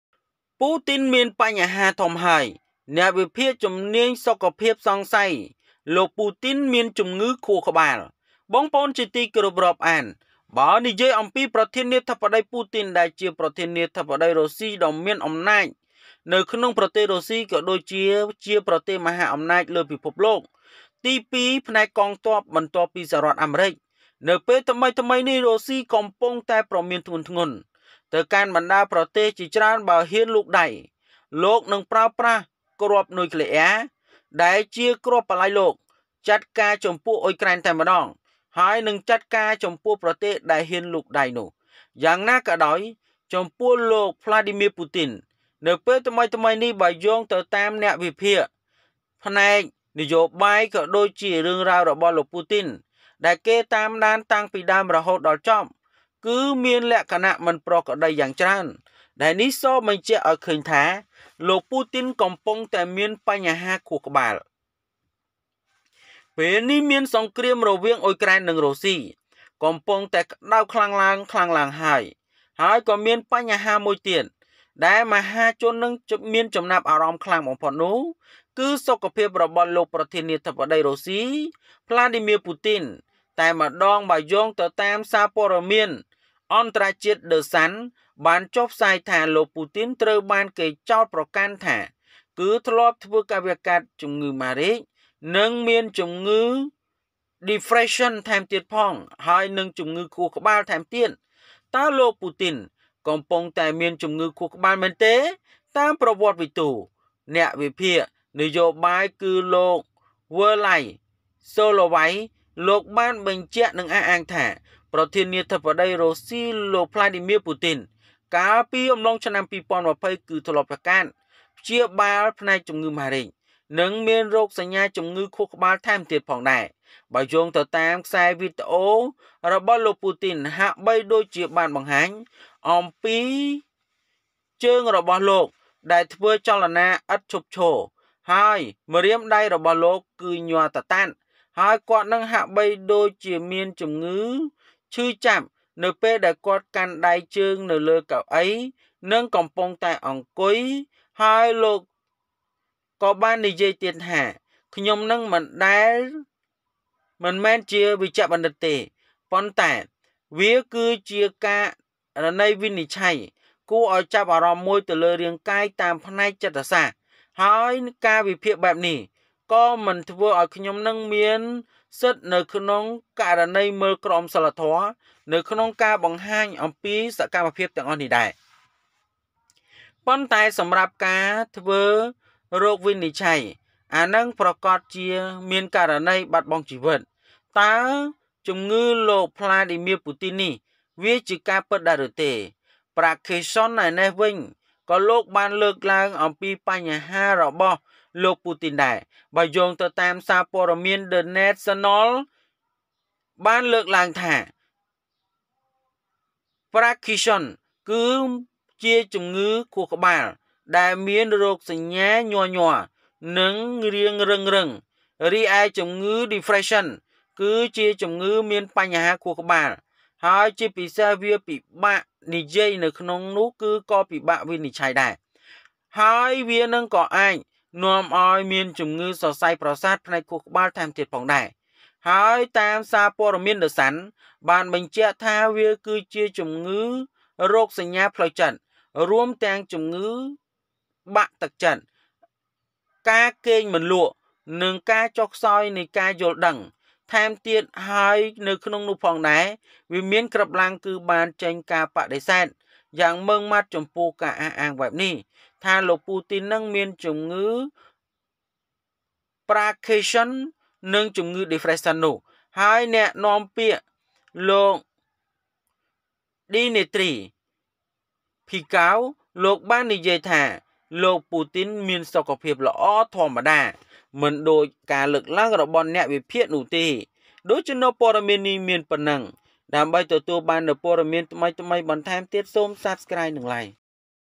ពូទីនបញ្ហាធំហើយ!! អ្នកវិភាគជំនាញសុខភាពសង្ស័យលោក ពូទីន មានជំងឺ
ពូទីនបញ្ហាធំហើយ!! អ្នកវិភាគជំនាញសុខភាពសង្ស័យលោក ពូទីន មានជំងឺ Putin is a big problem !!  Health analysts suspect Putin sick.